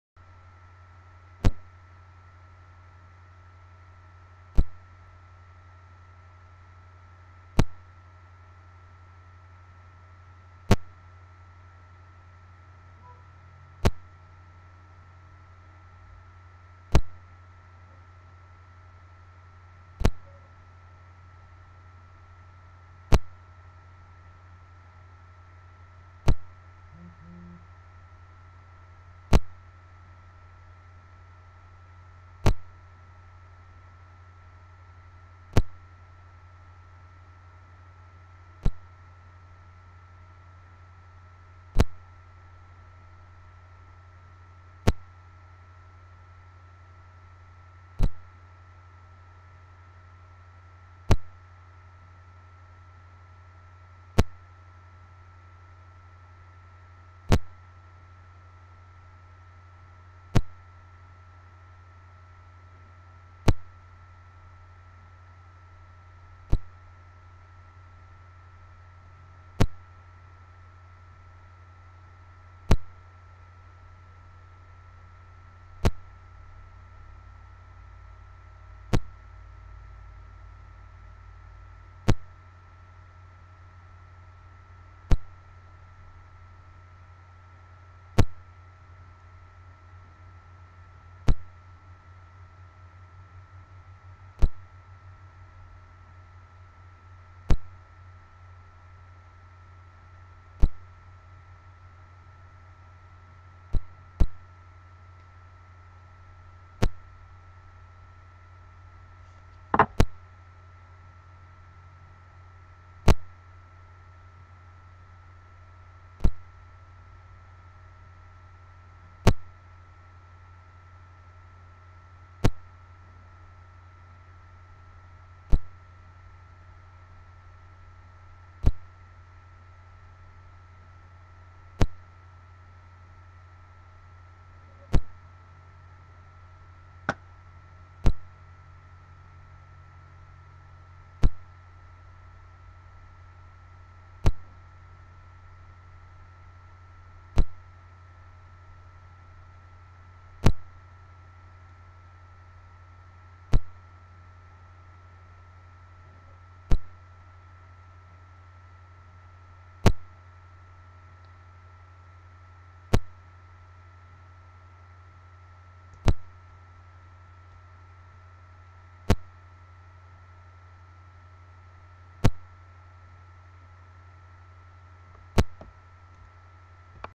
Interview Summary of Alex Smith
Deaf Culture and History Assignment
Alex Smith is my good friend!!